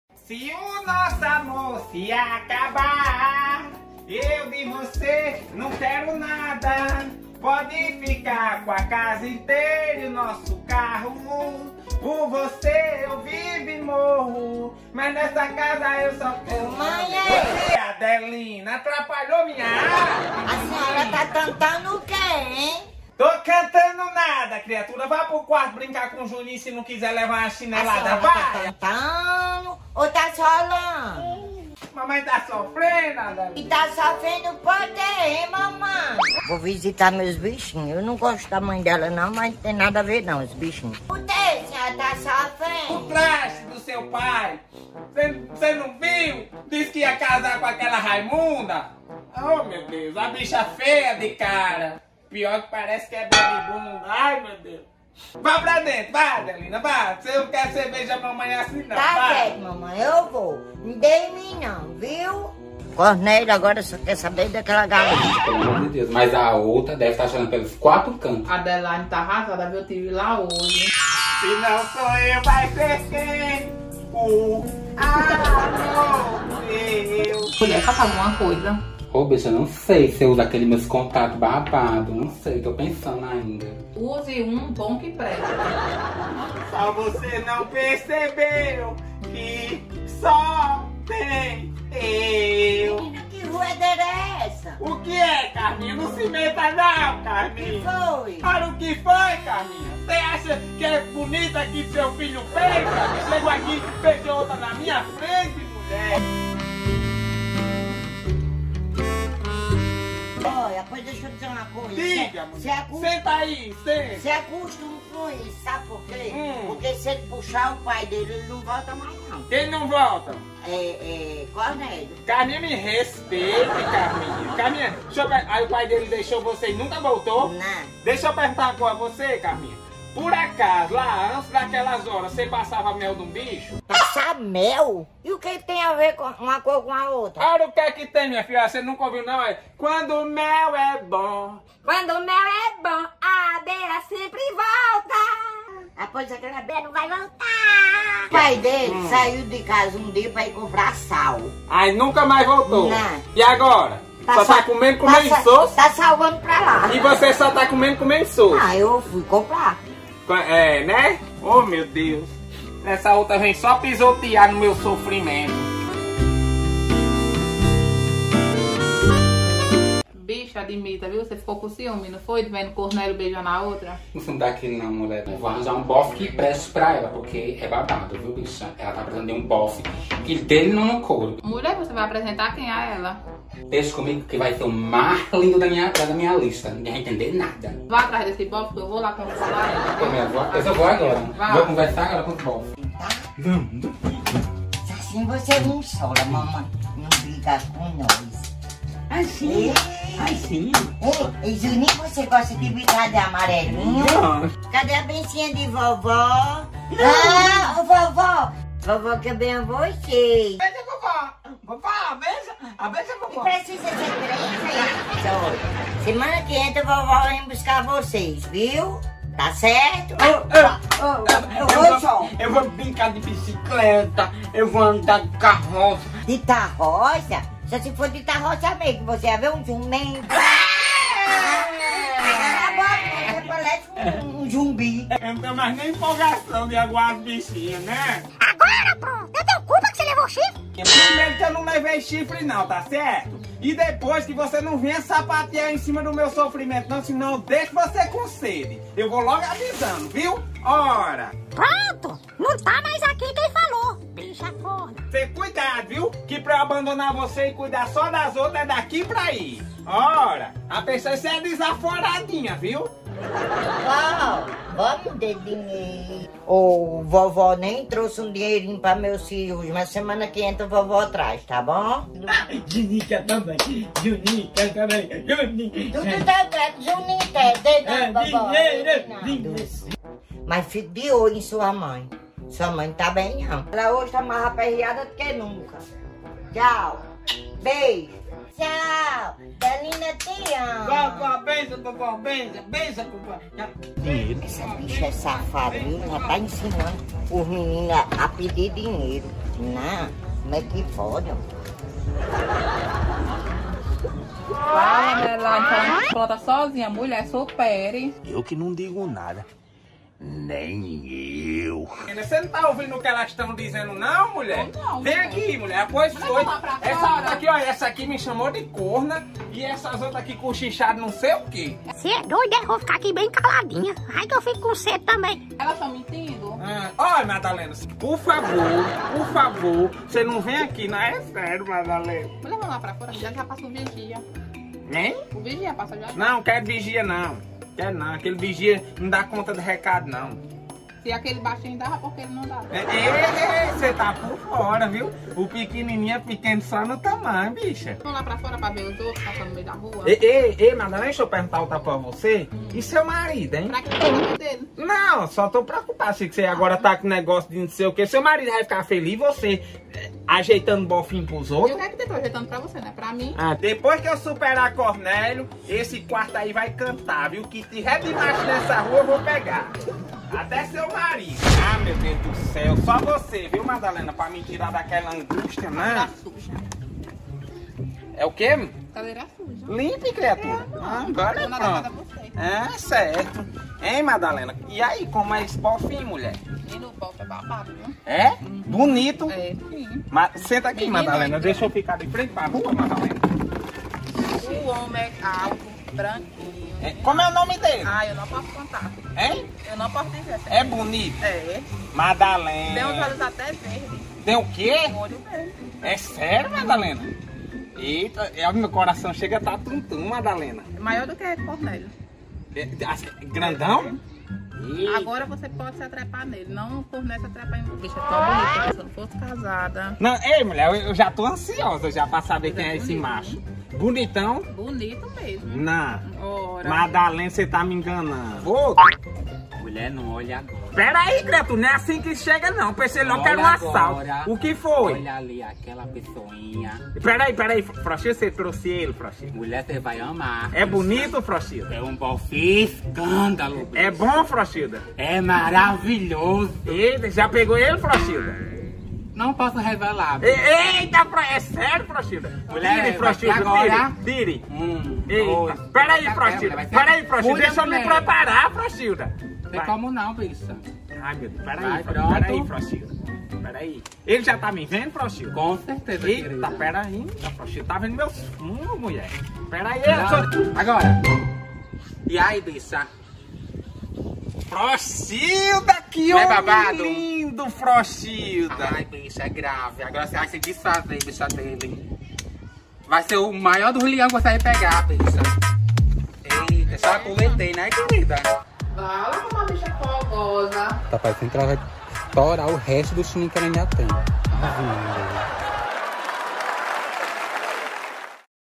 SOFRENCIA DE ADELAIDE E SURPRESA NO FINAL: MINHA SOGRA É UMA CHATA EP 20 TEMPORADA 2
Vendo Adelaide sofrer pela decisão de Cornélio, Madalena e Froxilda tentam ajudar a amiga arrumando um novo crush.

Toda honra e toda glória seja dada ao Senhor

Nos procure no insta!
Adelaide: kekedentaooficial
Adelina e Carminnha: donaireneoficial
Cornelio: bonnymossoro.of
Froxilda: vicky_millls
Madalena: Brenynhafilgueira

DIREÇÃO E ROTEIRO:
Keké Dentão

Imagens:
Keké Dentão/Ivodesouz4